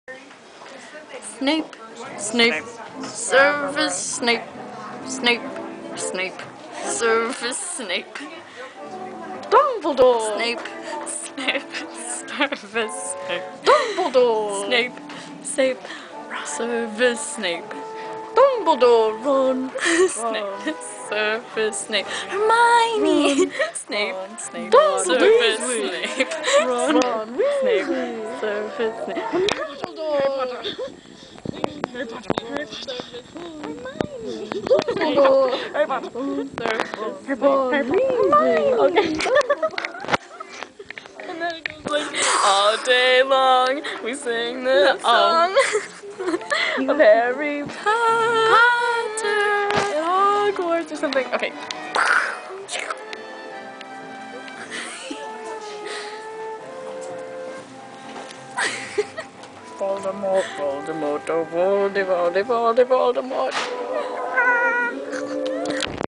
Harry Potter Puppet Pals
My sisters and I re-enacting HP puppet pals